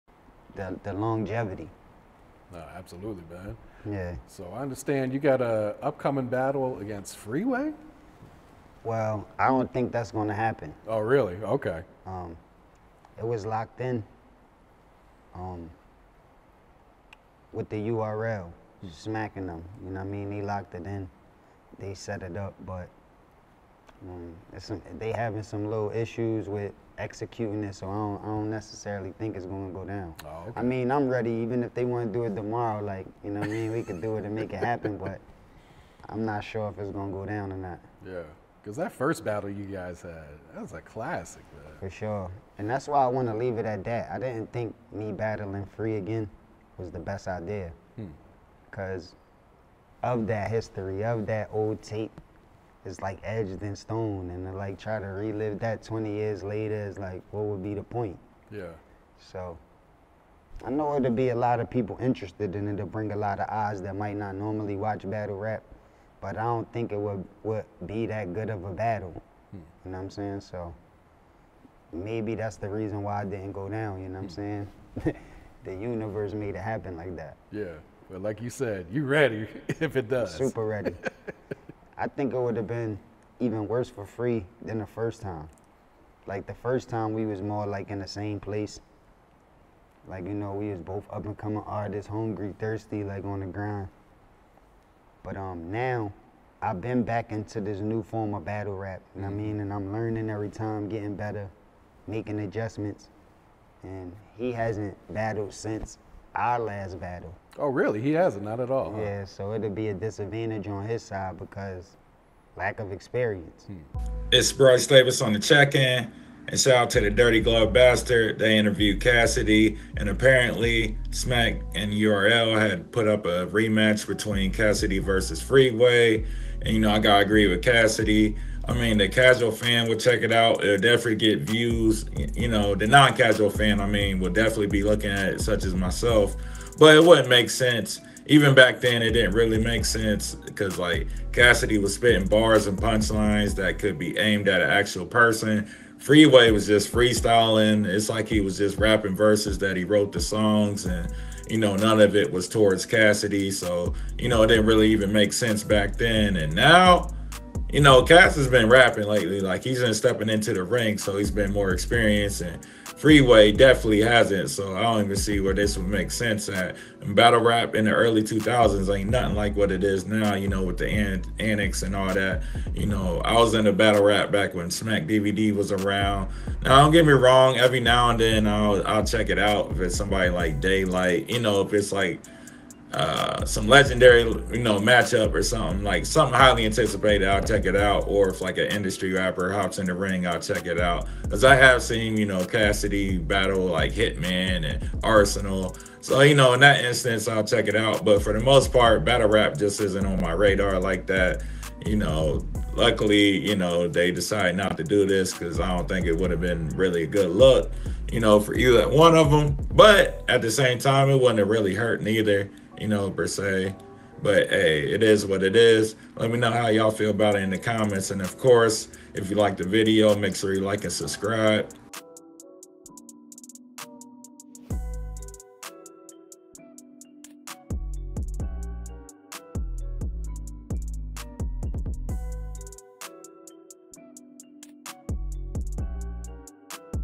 Cassidy Responds To Freeway Battle Rematch
Cassidy Addresses the rematch with Freeway that was set with Smack/URL in 2022 with the Dirty Glove Bastard.